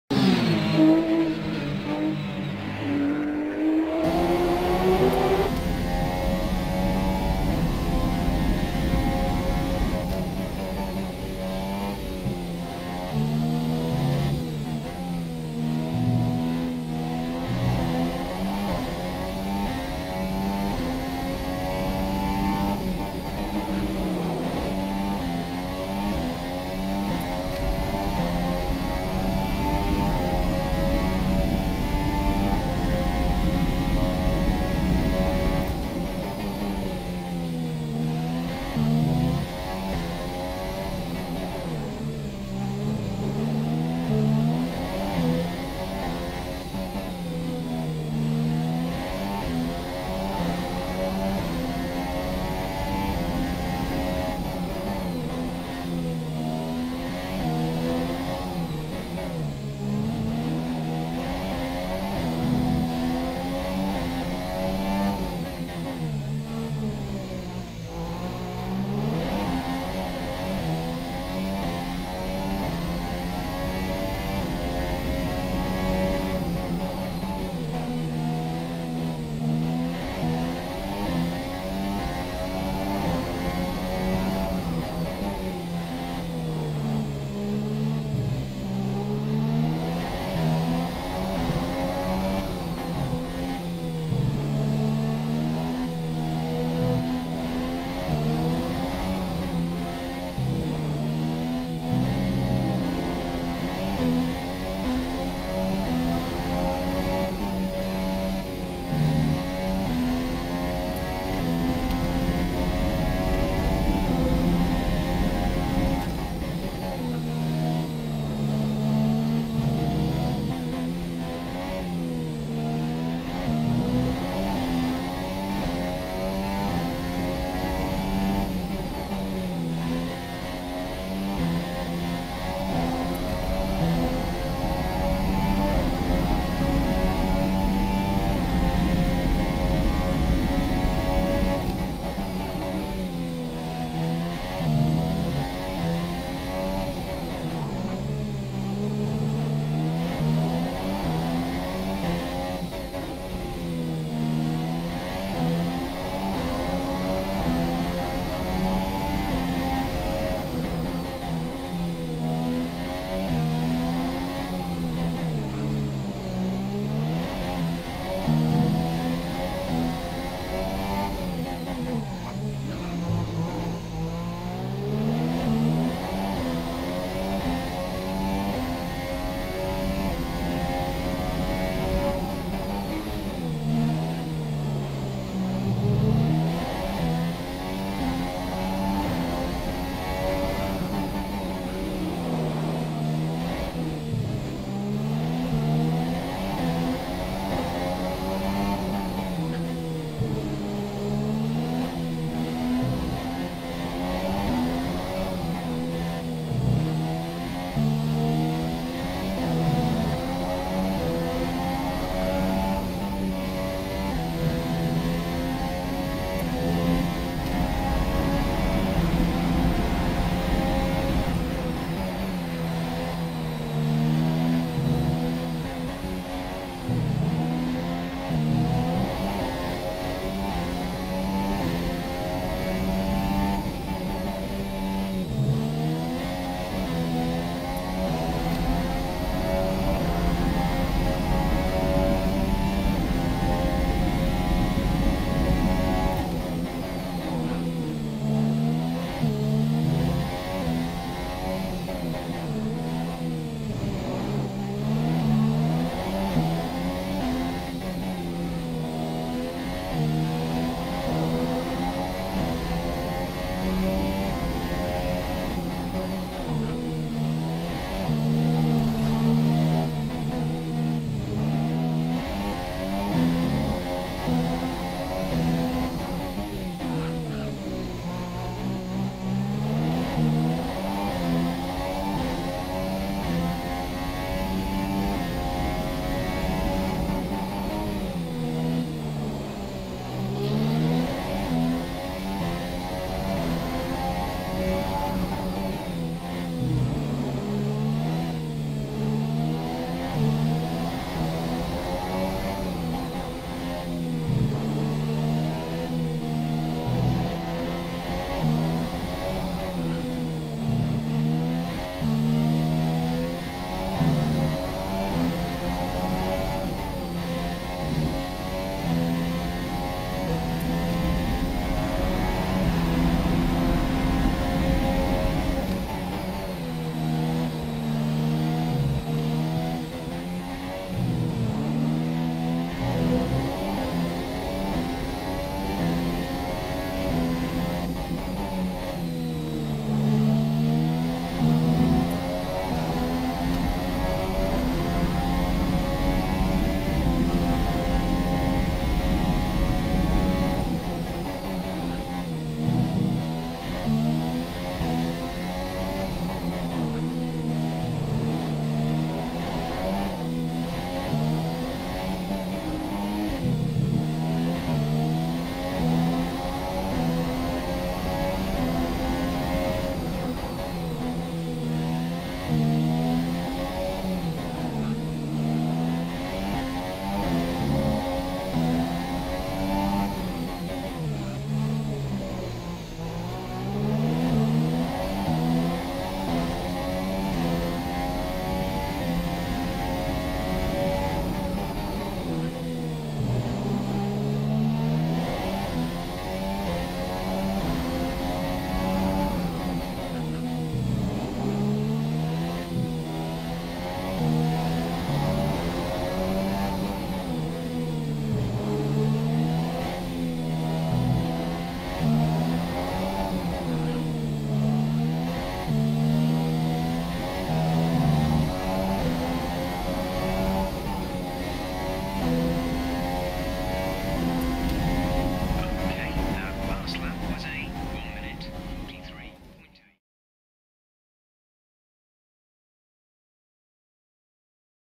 Getting to terms with Singapore
Never raced on Singapore circuit, so thought it was time to try it. Man this circuit is narrow and tricky. Real fun and nice quality in the night. Love it. 3 laps onboard.